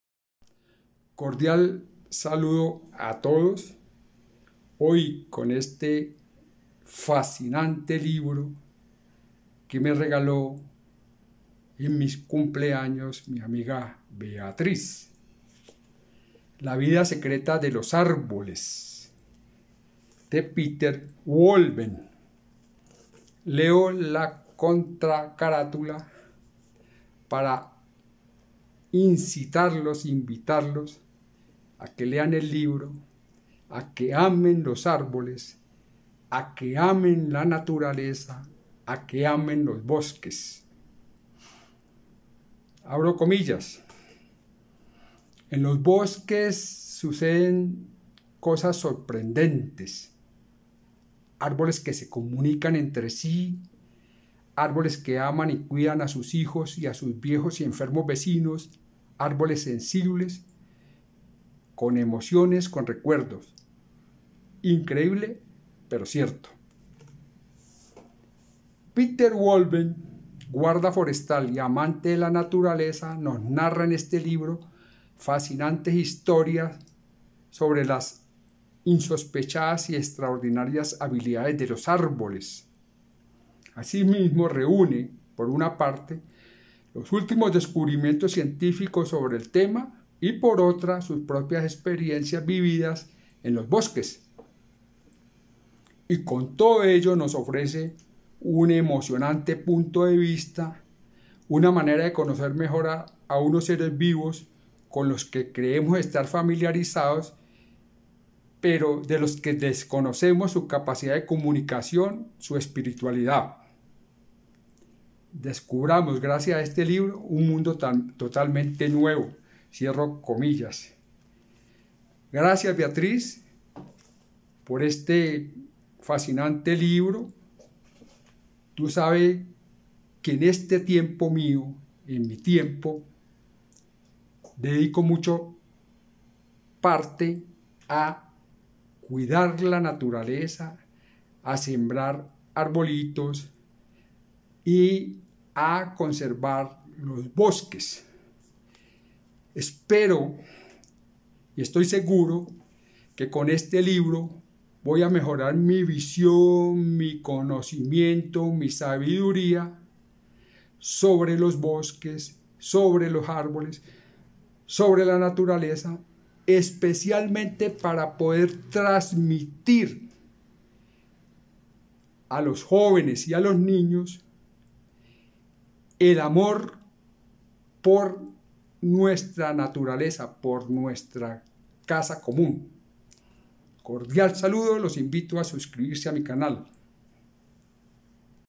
LA VIDA SECRETA DE LOS ÁRBOLES
Peter Wohlleben
En los bosques suceden cosas sorprendentes: árboles que se comunican entre sí, árboles que aman y cuidan a sus hijos y a sus viejos y enfermos vecinos; árboles sensibles, con emociones, con recuerdos... ¡ Increíble, pero cierto!